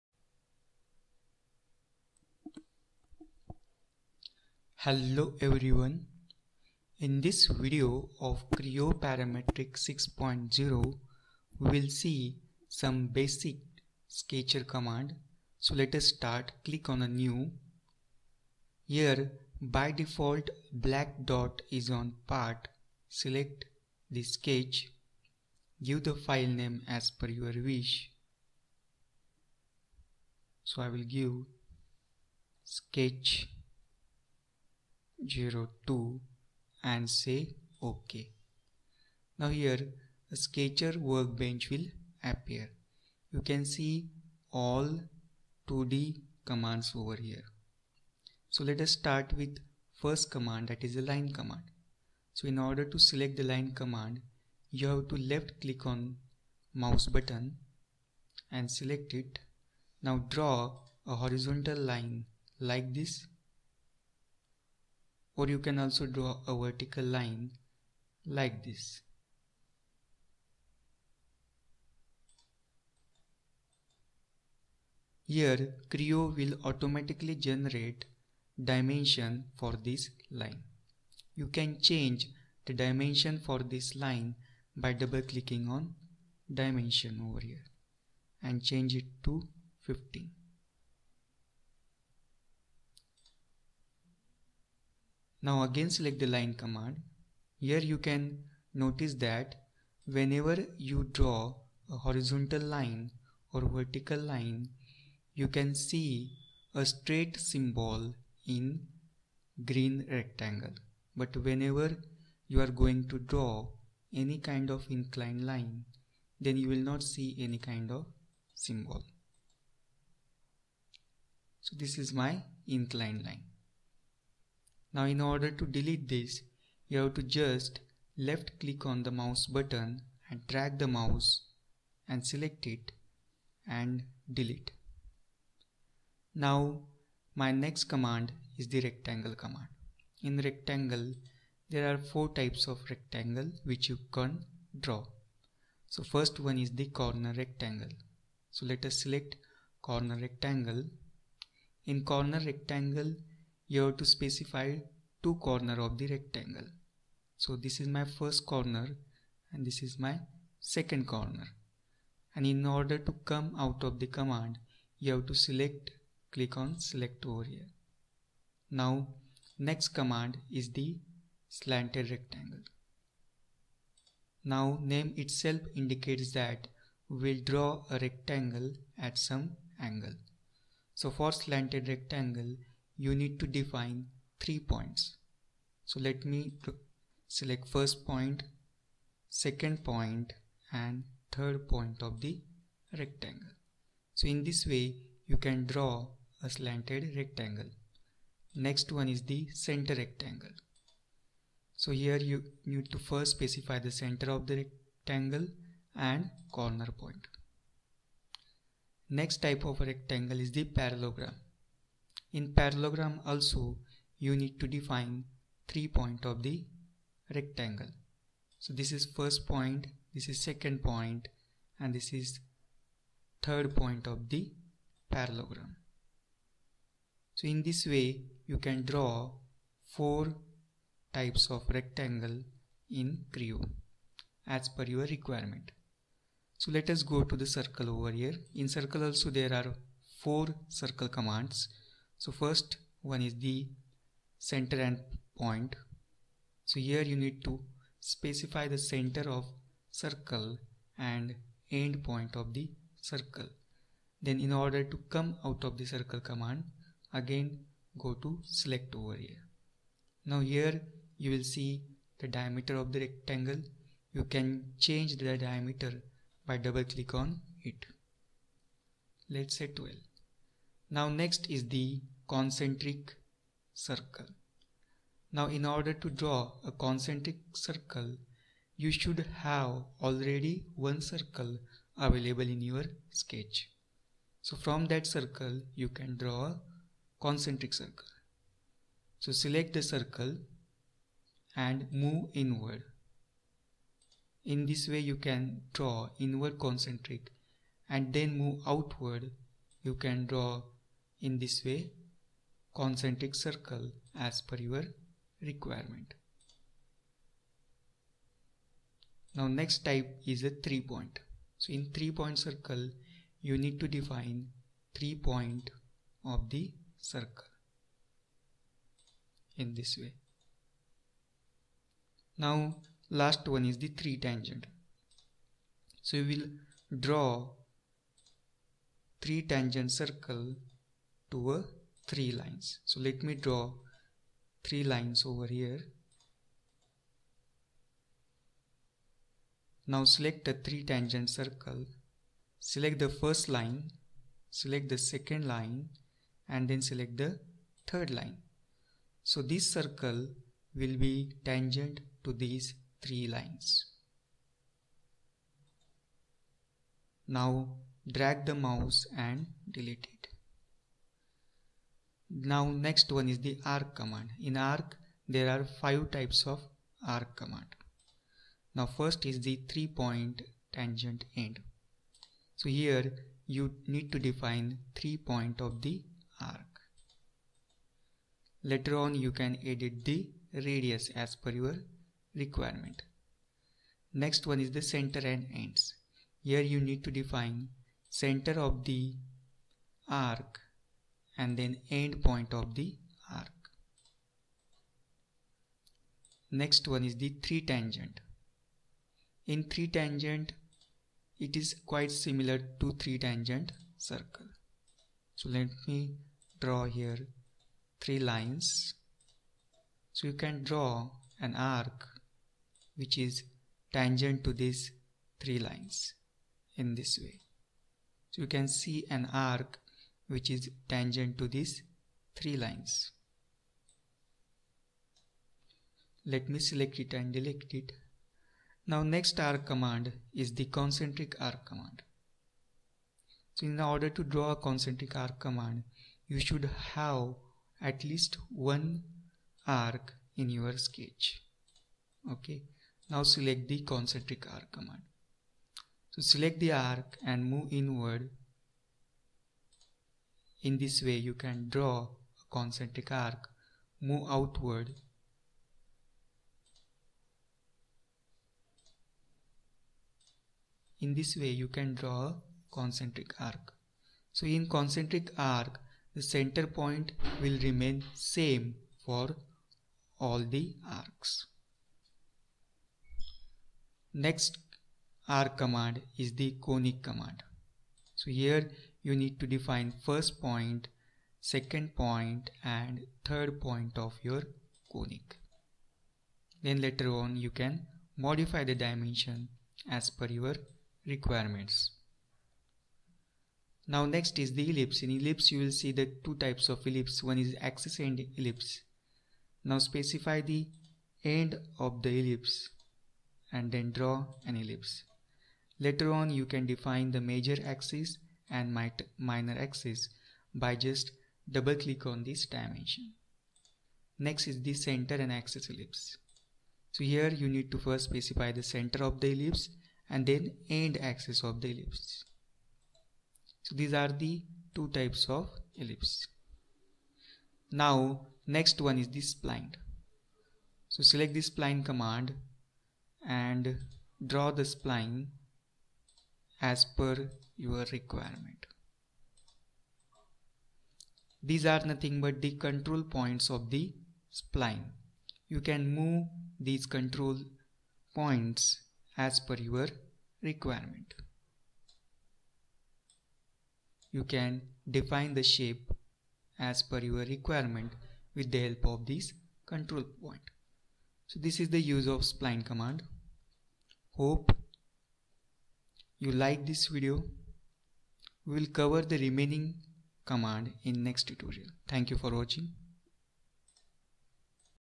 Creo 6.0 Tutorial for Beginners | Creo Sketch Tutorial.
in this video i have covered about creo 6.0 sketch tutorial for beginners.
sketch command covered in this video are line, rectangle, circle, ellipse,
spline, arc, fillet and chamfer.